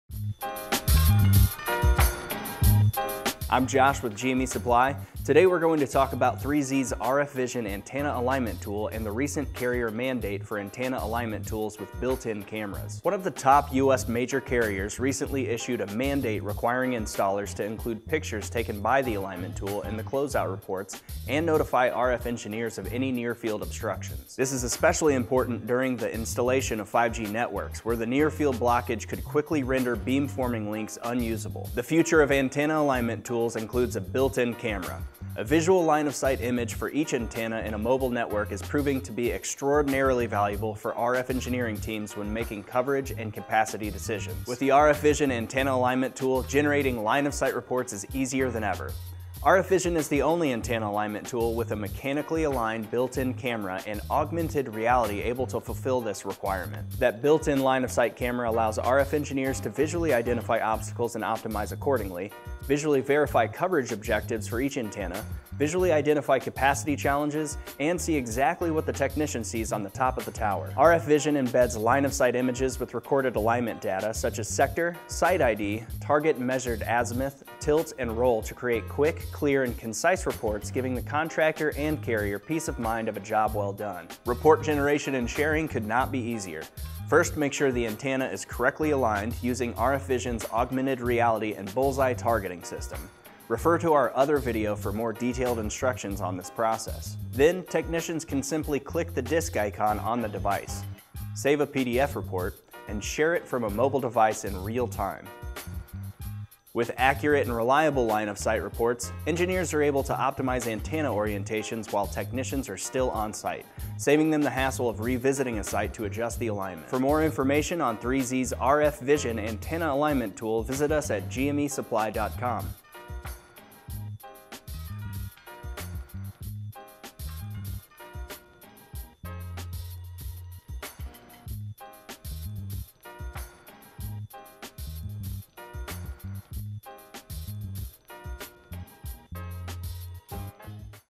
Quick and Easy Line of Sight Reports featuring 3Z Telecom
Comply with carrier mandated line of sight closeout reports with the 3Z RF Vision Antenna Alignment tool.

One of the top US major carriers recently issued a mandate requiring installers to include pictures taken by the alignment tool in the close-out reports and notify RF engineers of any near-field obstructions. The 3Z RF Vision is the only antenna alignment tool with a mechanically aligned, built-in camera and augmented reality able to fulfill this requirement.